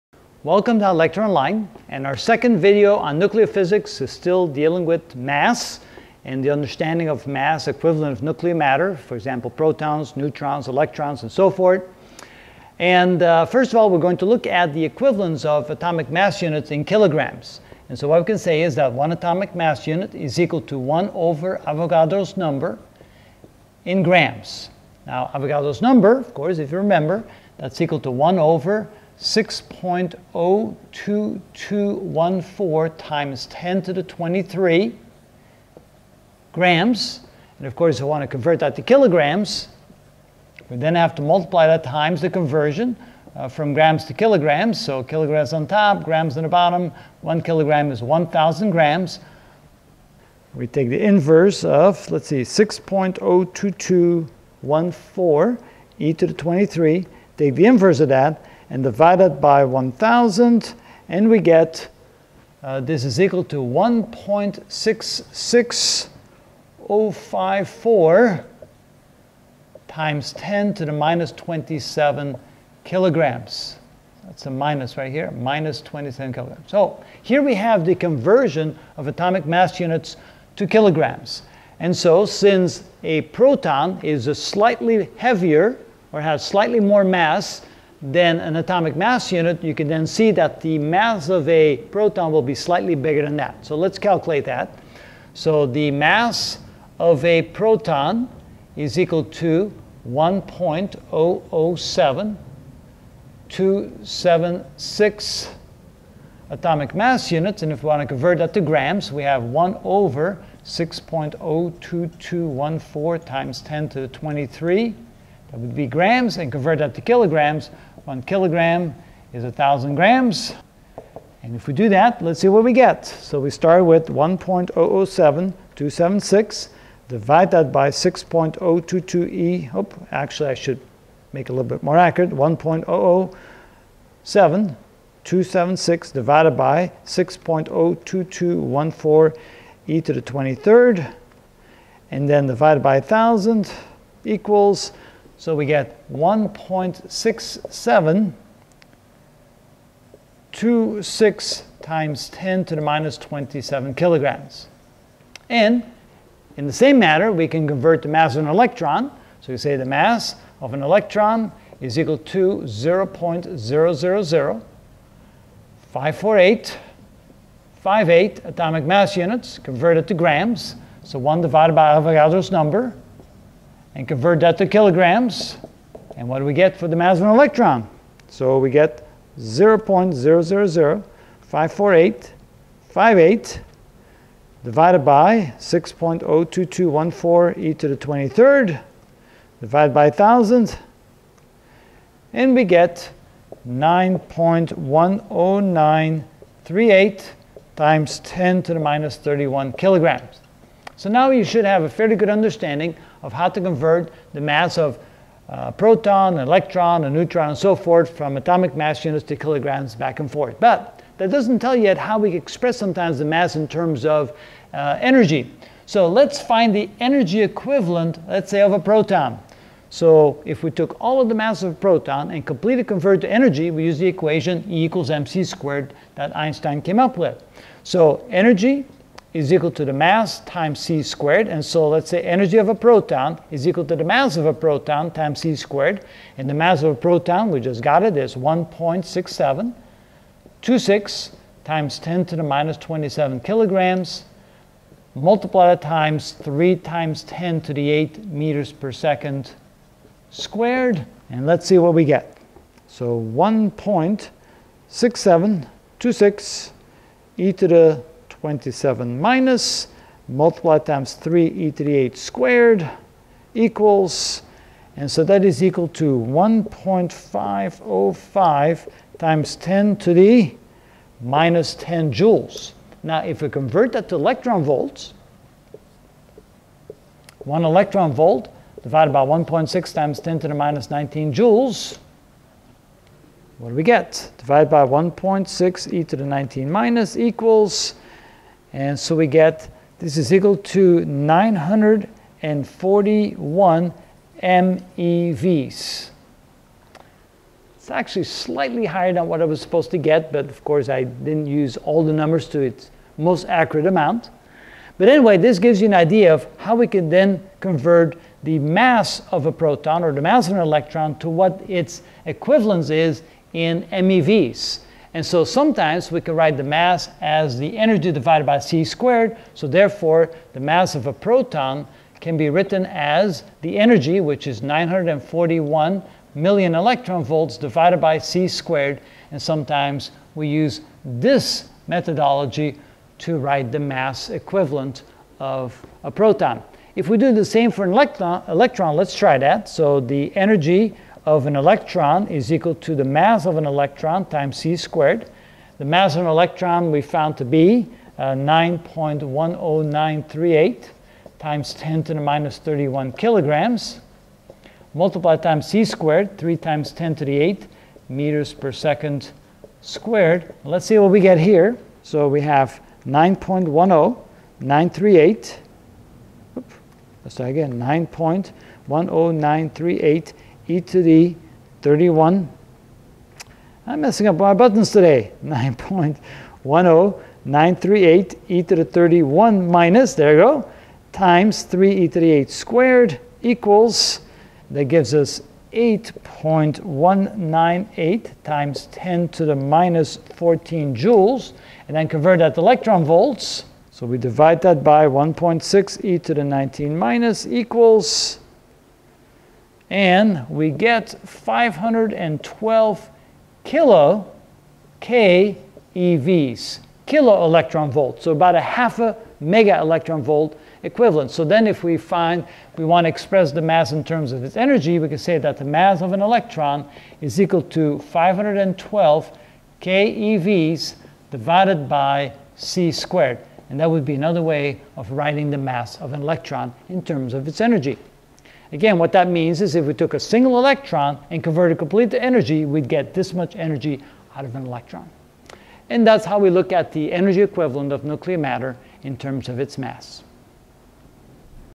Physics - Nuclear Physics (2 of 22) Energy Equivalent of Nuclear Matter
In this video I will show you how to find the energy equivalent of nuclear matter.